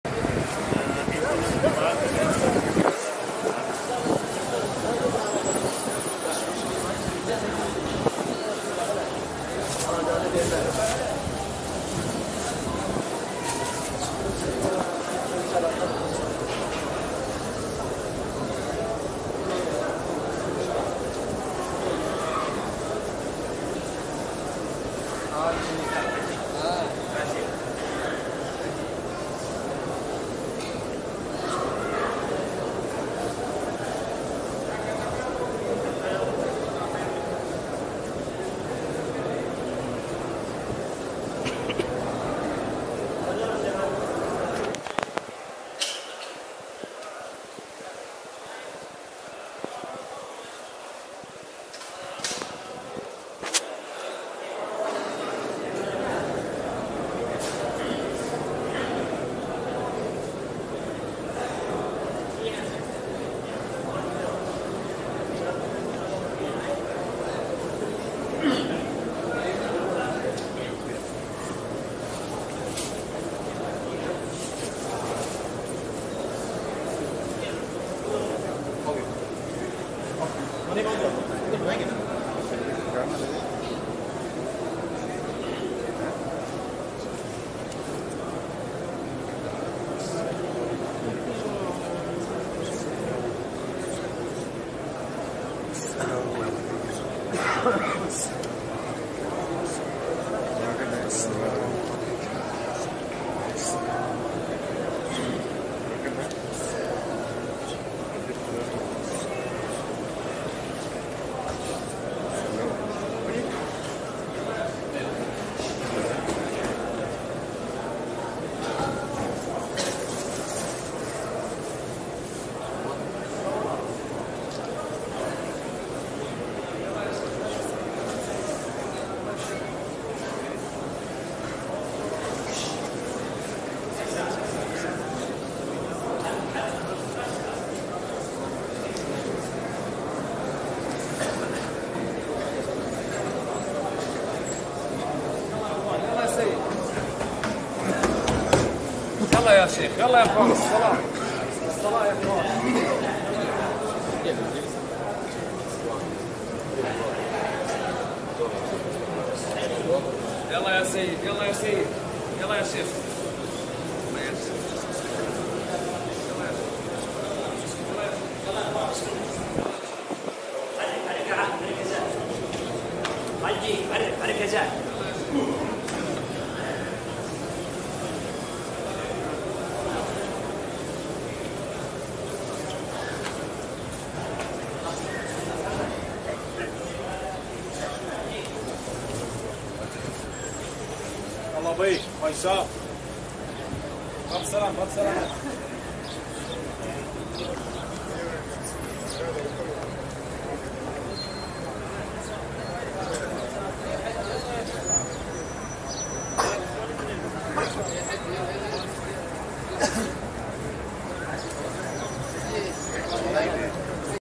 Visit of Roza-e-Rasool - Masjid Nabawi 360 6K Beautiful Film
This film is part of my rent Umrah visit 2019 for Saudi Arabia and visit to Masjid Nabawi. I try to capture a quick journey of visiting Roza-e-Rasool (PBUH). This 360 video is so peace full and remind me every mint spent there Alhmdolillah.

Please do not re-publish this video without my permission. This video is copy right 2020.